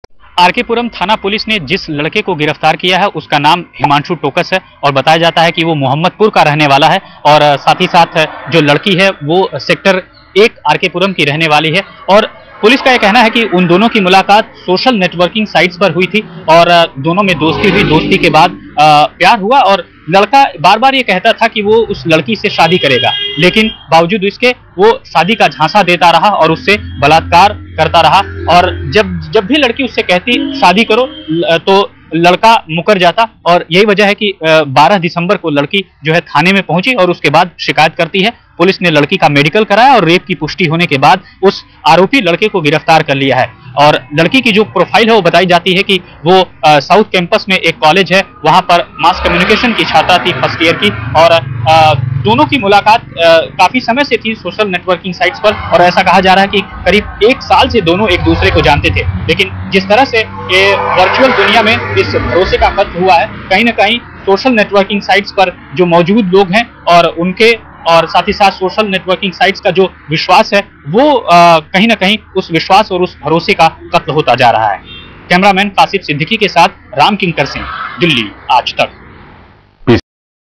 सोशल नेटवर्किंग साइट्स का साइड इफेक्ट
लड़की की सोशल नेटवर्किंग साइट के ज़रिेये एक छात्रा के साथ हुआ बलात्कार।